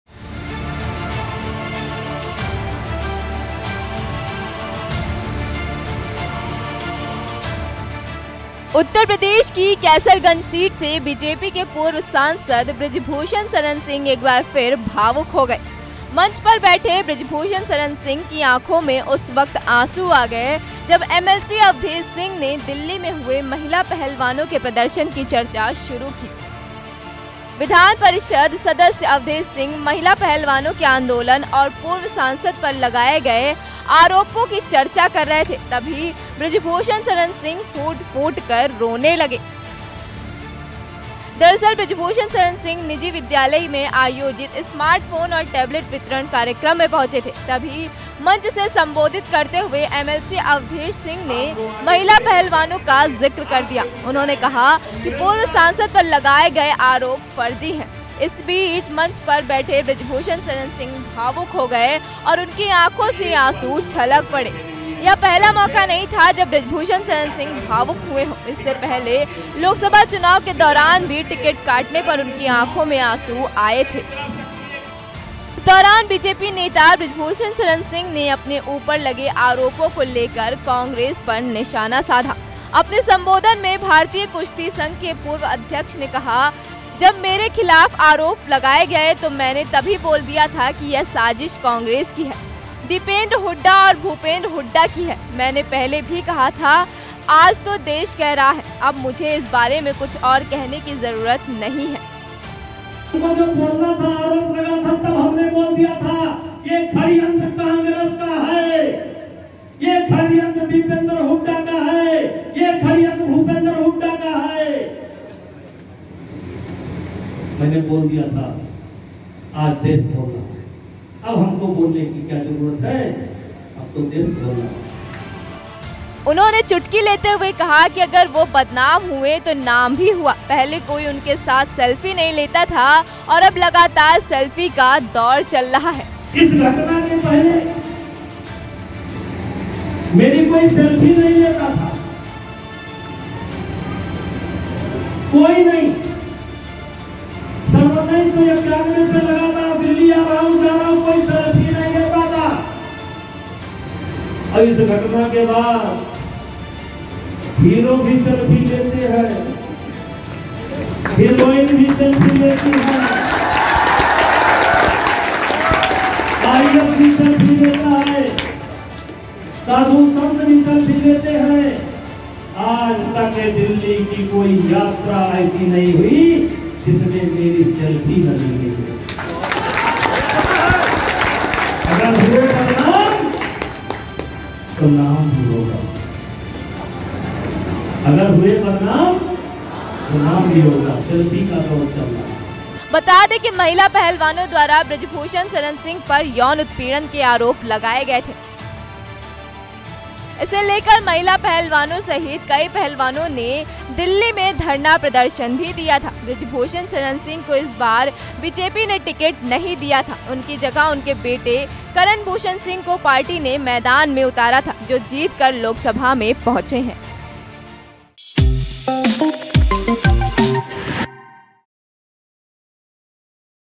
UP Politics: इस बार ऐसा क्या हुआ कि मंच पर फिर फूट फूट कर रोए Brij Bhushan Sharan Singh
उत्तर प्रदेश की कैसरगंज सीट से बीजेपी के पूर्व सांसद बृजभूषण शरण सिंह एक बार फिर भावुक हो गए. मंच पर बैठे बृजभूषण शरण सिंह के आंखों में उस वक्त आंसू आ गए जब एमएलसी अवधेश सिंह ने दिल्ली में हुए महिला पहलवानों के प्रदर्शन की चर्चा शुरू की. विधान परिषद सदस्य अवधेश सिंह महिला पहलवानों के आंदोलन और पूर्व सांसद पर लगाए गए आरोपों की चर्चा कर रहे थे, तभी बृजभूषण शरण सिंह फूट-फूटकर रोने लगे.
दरअसल, बृजभूषण शरण सिंह निजी विद्यालय में आयोजित स्मार्टफोन और टेबलेट वितरण कार्यकर्म में पहुंचे थे. तभी मंच से सम्बोधित करते हुए एमएलसी अवधेश सिंह ने महिला पहलवानों का जिक्र कर दिया. उन्होंने कहा कि पूर्व सांसद पर लगाए गए आरोप फर्जी हैं. इस बीच मंच पर बैठे बृजभूषण शरण सिंह भावुक हो गए और उनके आंखों से आंसू छलक पड़े. यह पहला मौका नहीं था जब बृजभूषण शरण सिंह भावुक हुए हो